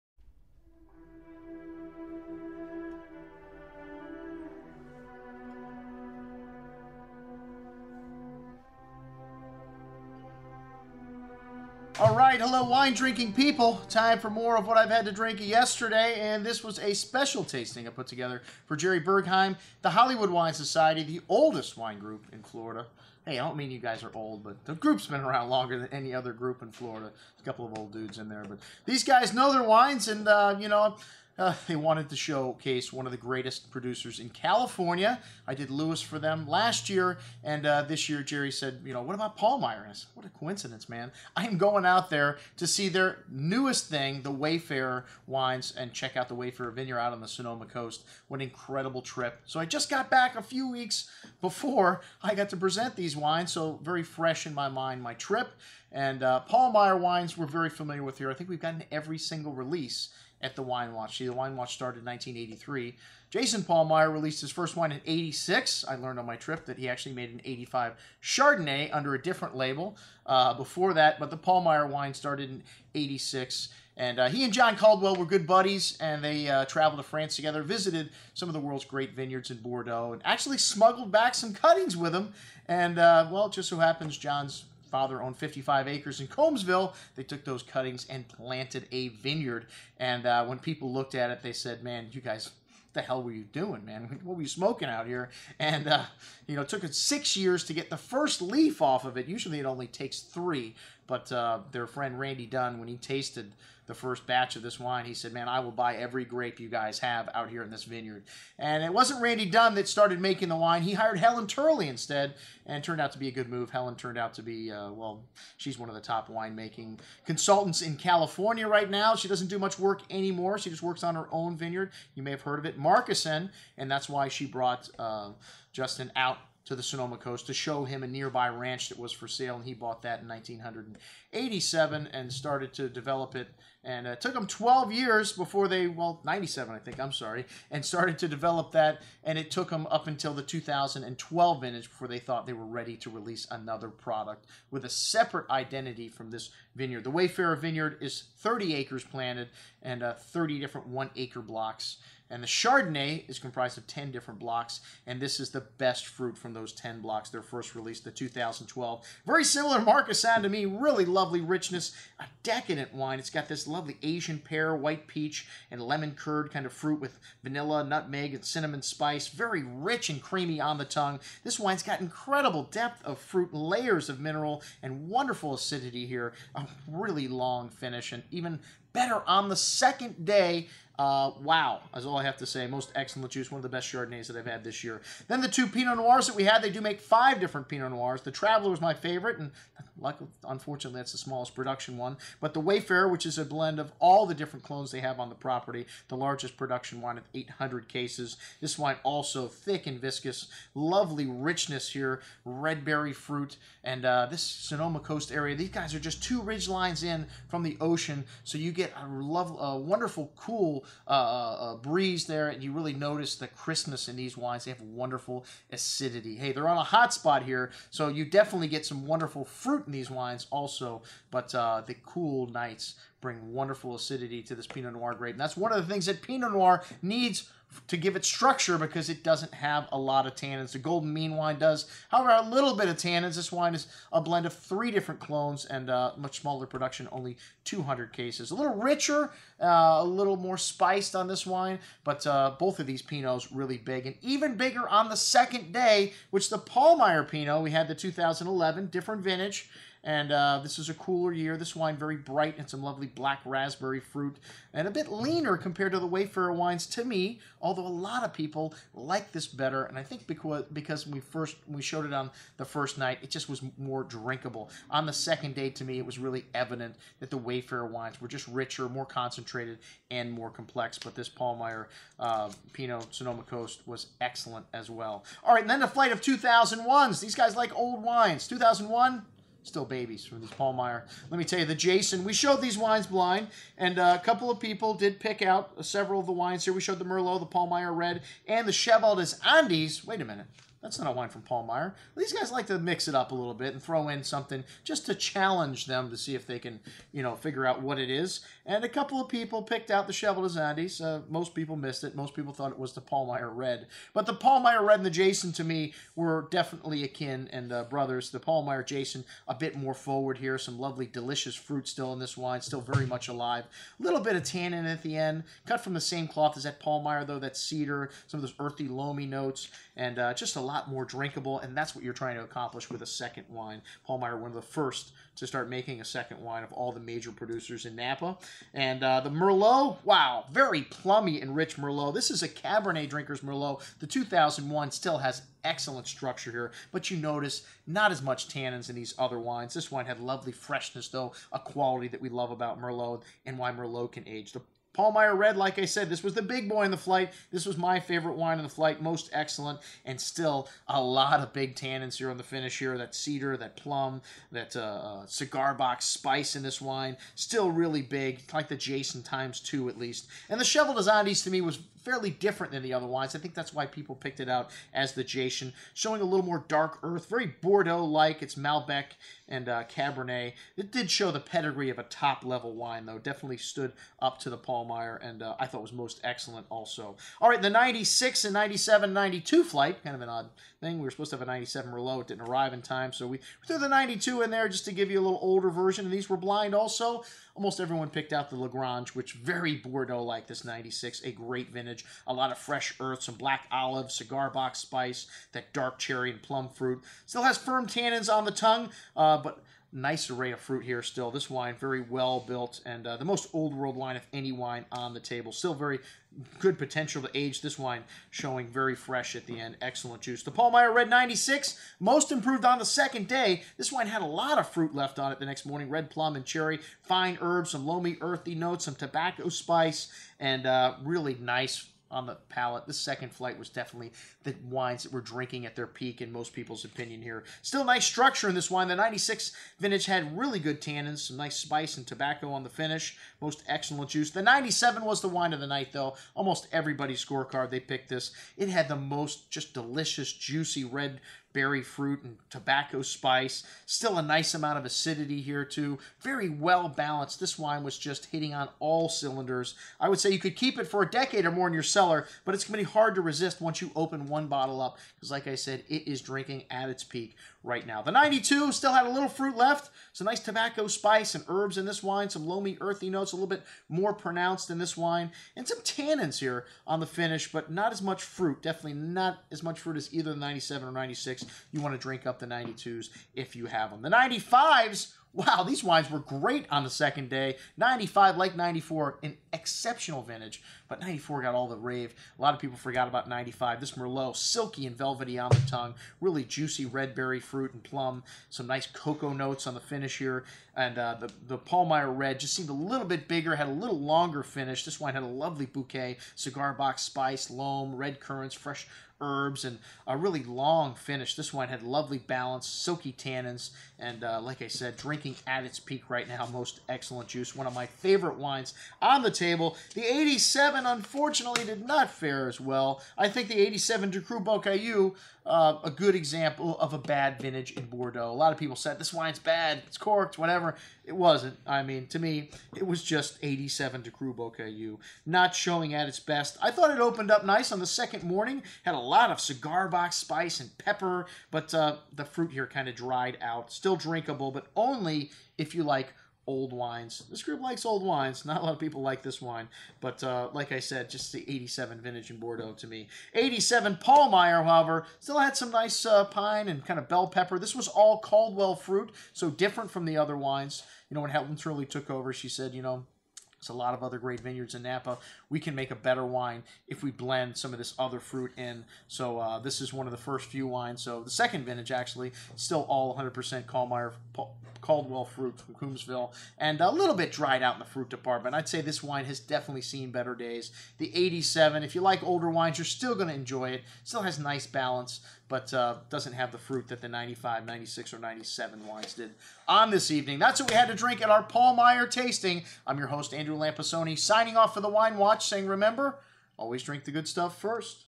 What I Drank Yesterday- Pahlmeyer & Wayfarer Review
They have been on a constant path of improvement at Pahlmeyer winery and when Jayson Pahlmeyer started out in 1986, he had no winery and no vineyards but was determined to make one of California’s best wines and one that would stand up to the best of those made in Bordeaux, France and the rest of the world.

I was happy to accept the Pahlmeyer family’s invitation to visit the Wayfarer Vineyard in August of this year and it worked out perfectly for me as I was already out on the west coast for the Washington State Wine Auction. It was great to visit the Fort Ross Seaview appellation with one of the state’s best ocean side resorts Tamber Cove. This place was a throwback in time and one of the most unique establishments that I have ever spent time and the only place to have a group stay that is coming out to visit the vineyards out here on the true Sonoma Coast.
They are on 3 main ridges they used three different root stocks because of the different flavor profiles because of the temperatures in the vineyards. They have several different clones on the property and these are the first releases from this exciting new label from this extreme growing region.
The first three wines are now released from this exciting new project from Pahlmeyer winery!!